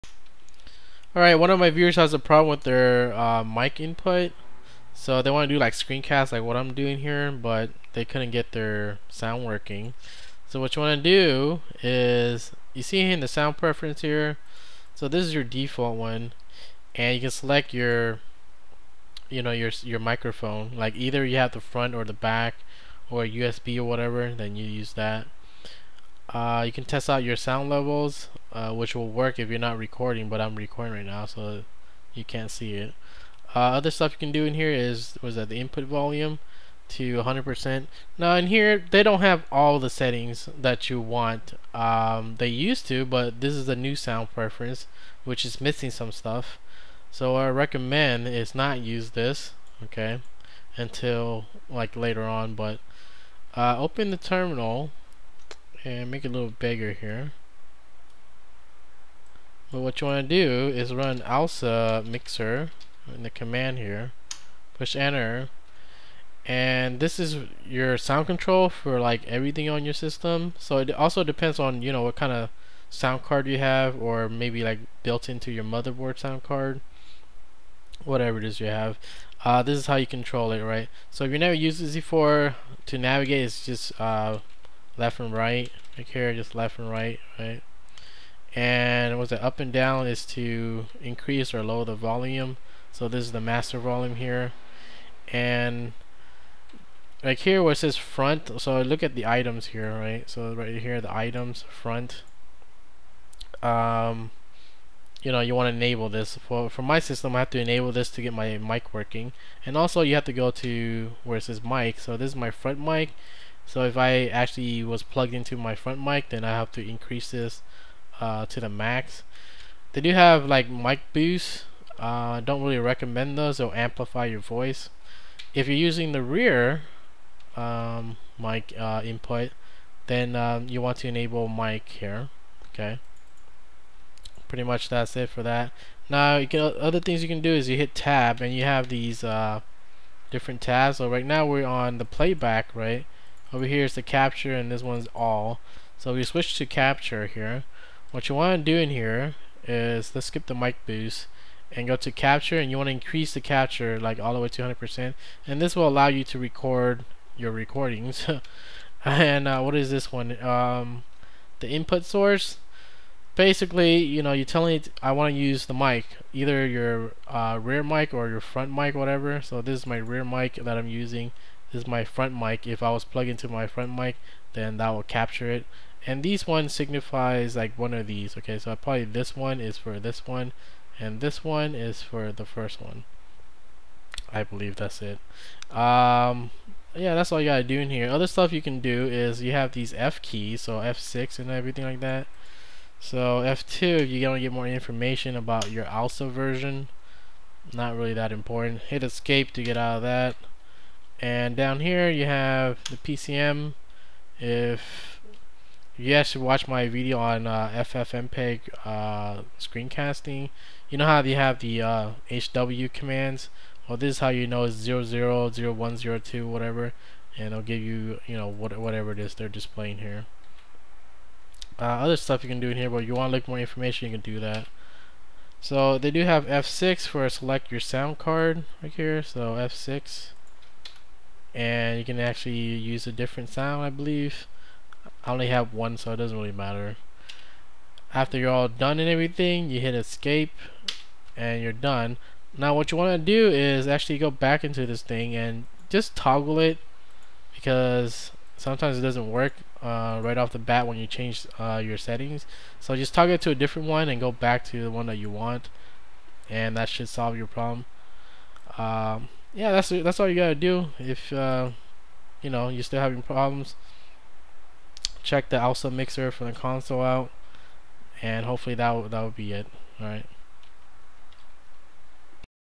Alsamixer - Ncurses ALSA Sound Control - Ubuntu 10.10
alsamixer - soundcard mixer for ALSA soundcard driver, with ncurses interface